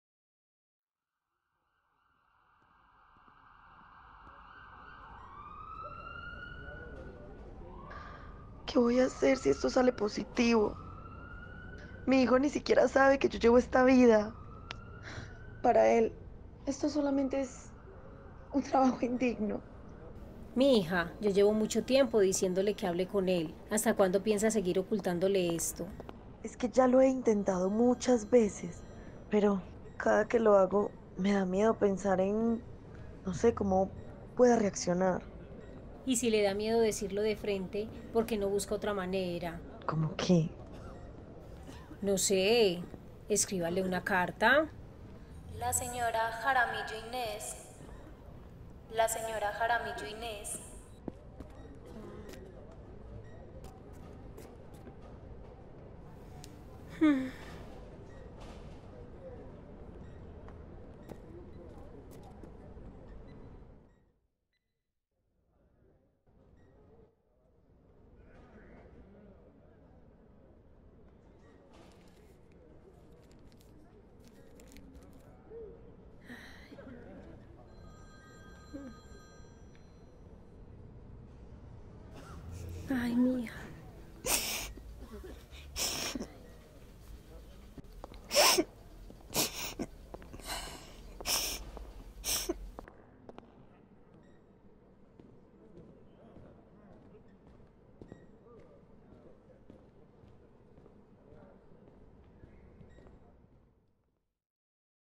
Teaser - La Carta (4K)
Ejercicio práctico para la entrega final de la asignatura de Taller Central I

Instituto Tecnológico Metropolitano
Medellín - 2020